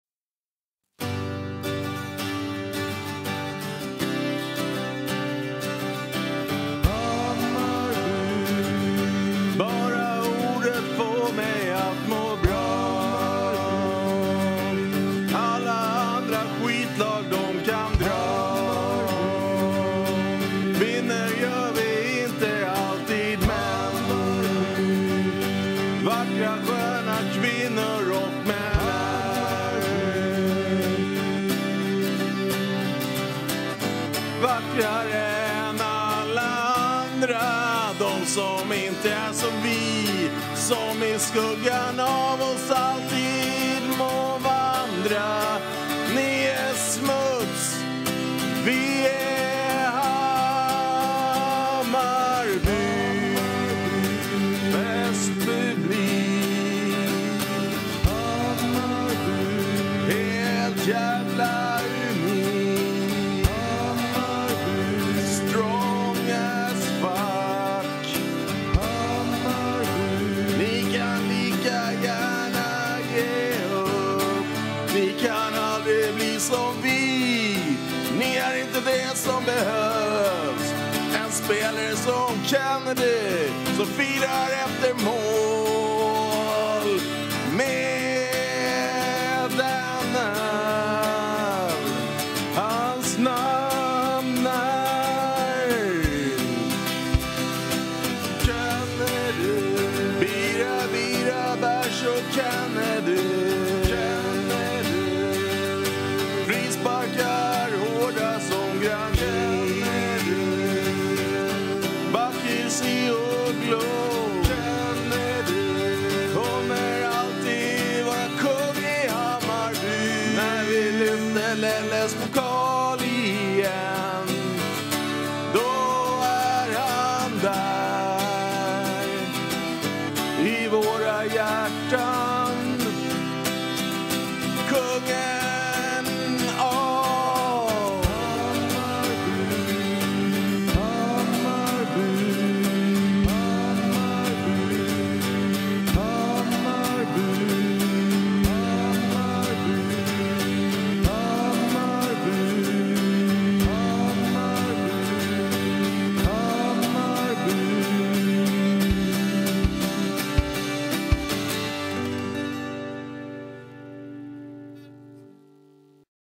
HAMMARBY ÄR
En hyllning till Hammarby och Kennedy som gör sin sista säsong i Hammarbytröjan.